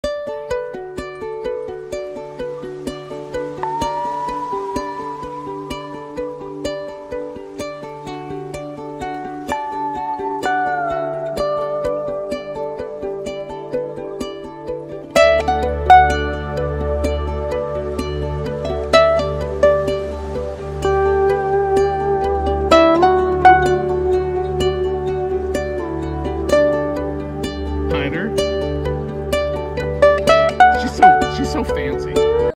Loving our ORU Lake foldable kayaks.
We were so excited to get our new folding ORU Lake Kayaks‼️

We have been very happy with how the ORU Kayaks track in the water, even in windy conditions. The Lake model has been very stable for us as we’ve now used them a handful of times.

The ORU Kayaks have been more convenient than our old inflatable kayak.

We can honestly say that it can be assembled in less than two minutes since I have timed Rich assembling the kayaks.  😄

With our inflatable kayak we found that we were not using them as much as we wanted due to the hassle and time it took to inflate and assemble our old kayak.

The ORU Kayaks have been more convenient than our old inflatable kayak. The Lake model has been very stable for us as we’ve now used them a handful of times.